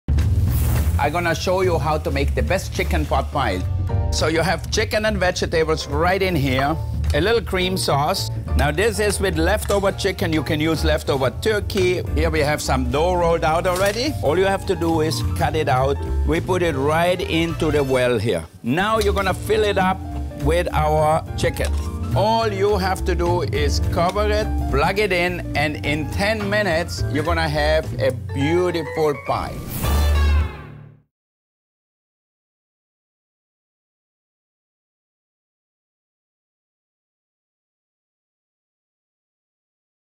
Wolfgang Puck's Chicken Pot Pie - Shoppable Video
Chef Wolfgang Puck shows you how-to create the perfect Chicken Pot Pie at home this winter!